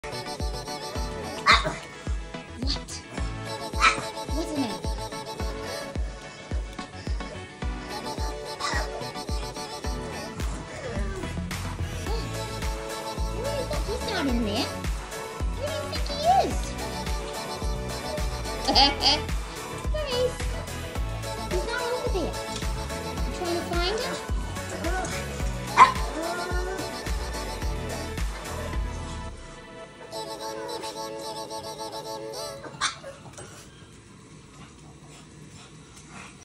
Dog reaction to tv show! Keep watching!😂Looks high and low￼!
Baby Grace the pug loves her TV show and today she was looking for that squirrel!